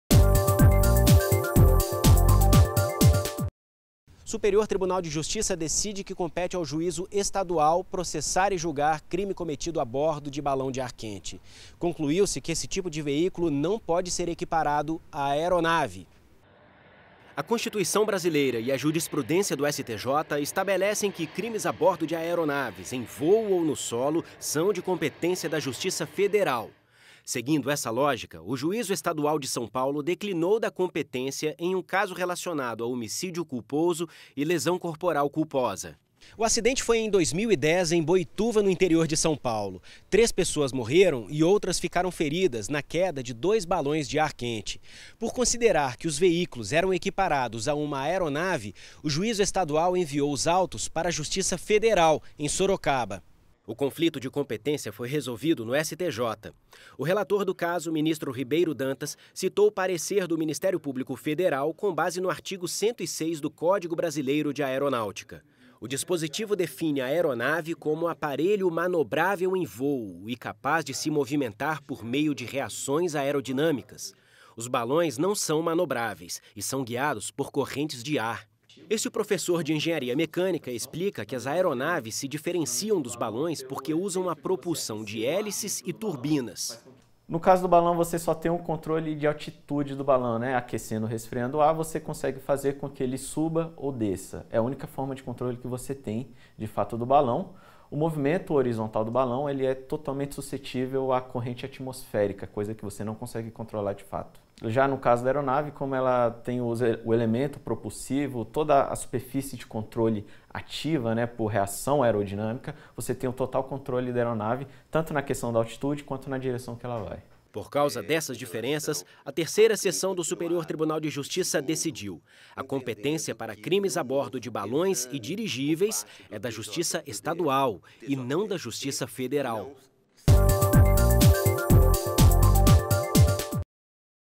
Clique STJ - Crime Balão (02/05/2019)
Superior Tribunal de Justiça decide que compete à Justiça Estadual processar e julgar crime ocorrido a bordo de balão de ar quente. O conflito negativo de competência teve início porque esse tipo de veículo não pode ser entendido como aeronave.